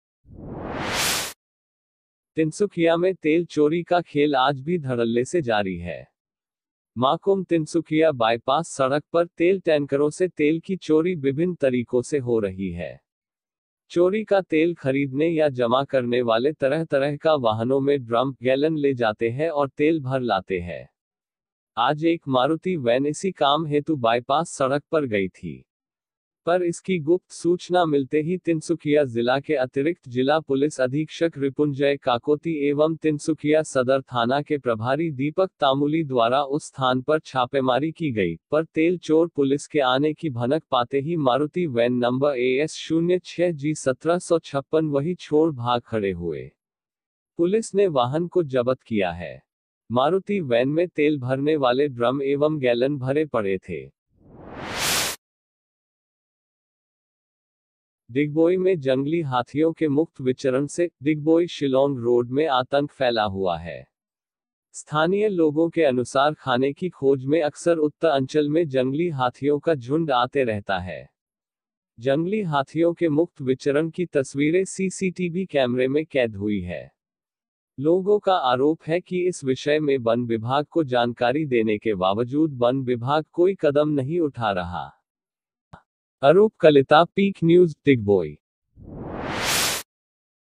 Peak news 12.02.2020 C
तिनसुकिया में तेल चोरों का वाहन जब्त
डिगबोई में  जंगली हाथियों का मुक्त विचरण